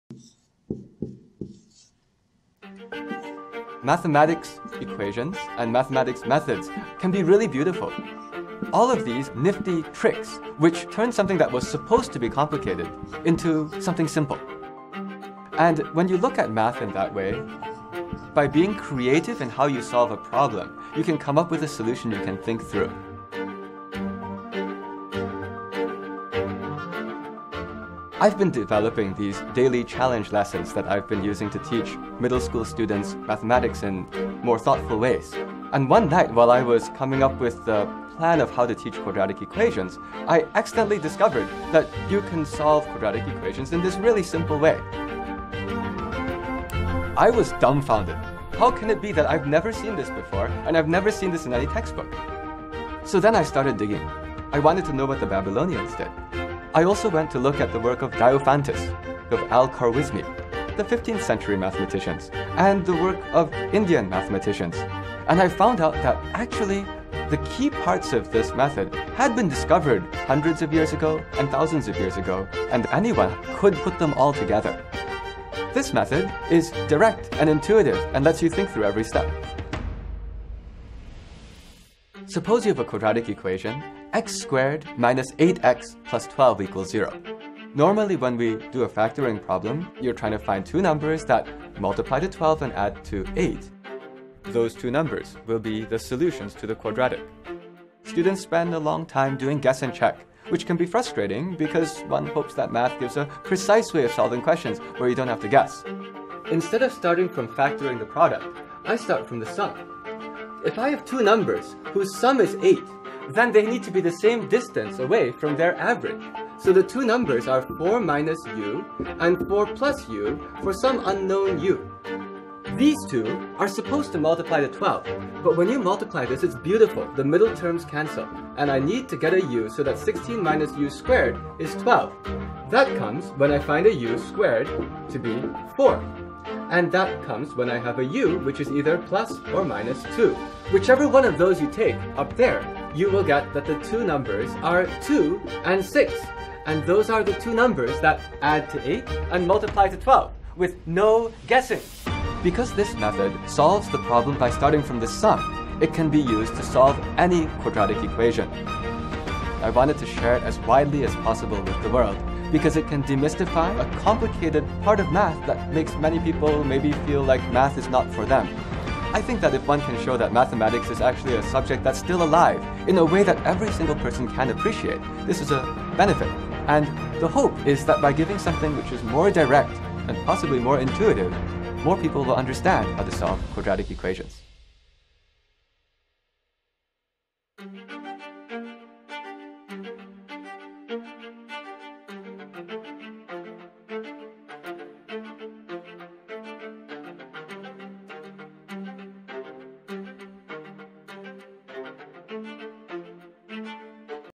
An Easier Method to Solve a Quadratic Equation
Learn to solve any quadratic equation without discriminant